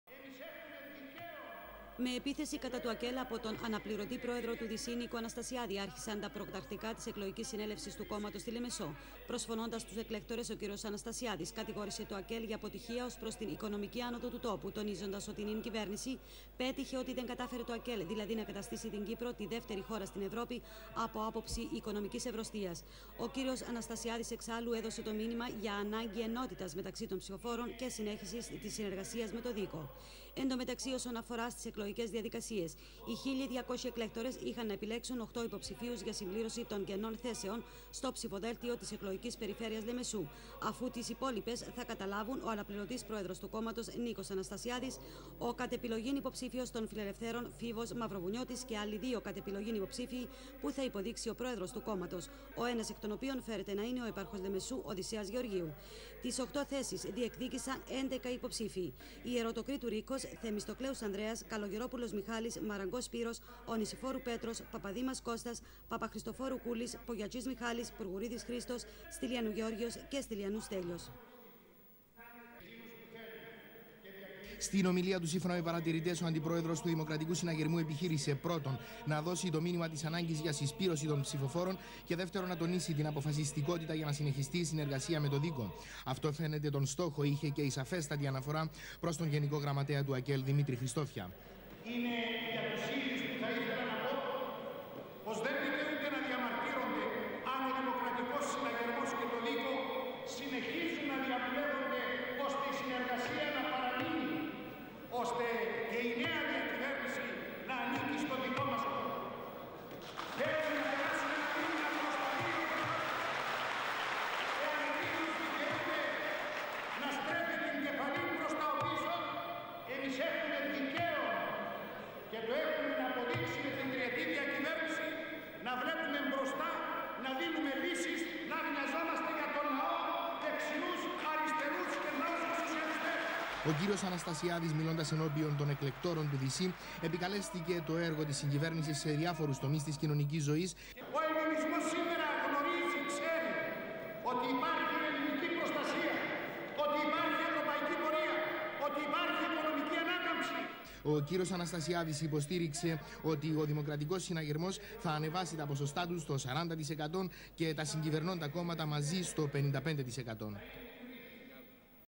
18 Φεβρουαρίου 1996 διαδικασία ανάδειξης υπoψηφίωv βoυλευτώv του ΔΗΣΥ για τη Λεμεσό.
18 Φεβρουαρίου 1996 διαδικασία ανάδειξης υπoψηφίωv βoυλευτώv του ΔΗΣΥ για τη Λεμεσό.
Ρεπορτάζ: Αννα Δημητρίου και Πάνος Ιγνατίου
Κάμερα: Κλείτος Ερωτοκρίτου
Μοντάζ: Ανδρέας Μιχαηλίδης
Αρχείο: Videoscope studio